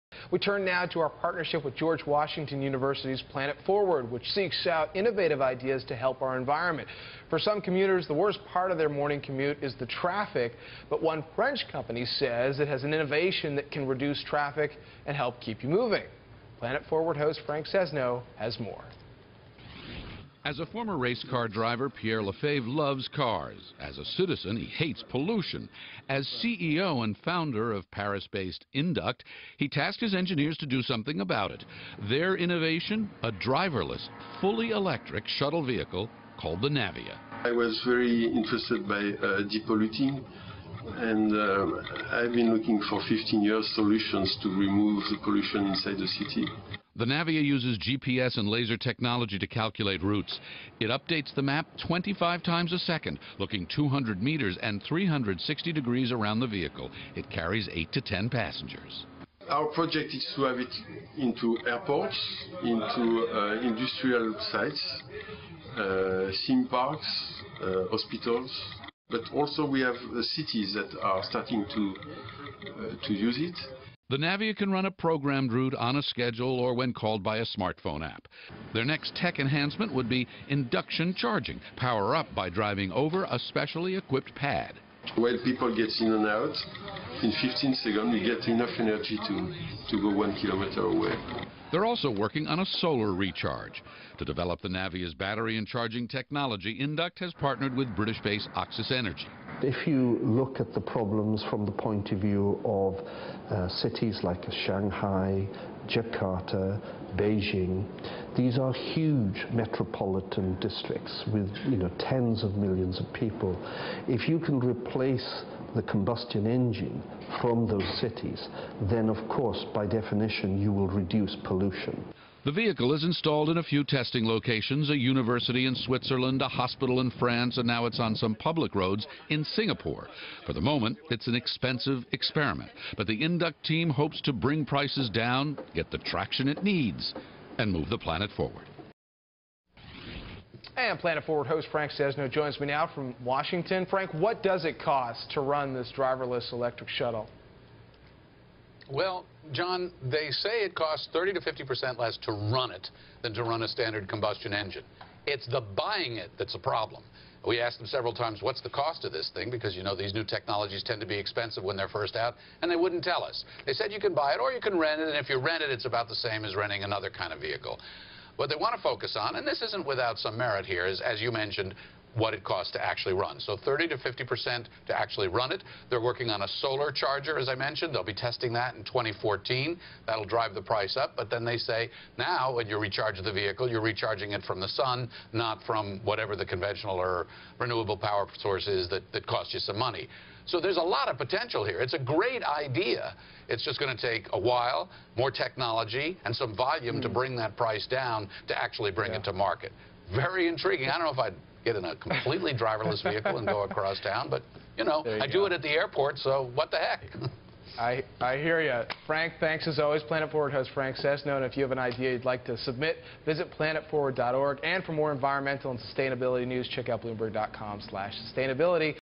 Go Green by Ditching the Driver
Sit back, relax and enjoy the ride in Navia, a driverless electronic shuttle invented by a former racecar driver that brings robotics to a whole new level through GPS and laser technology.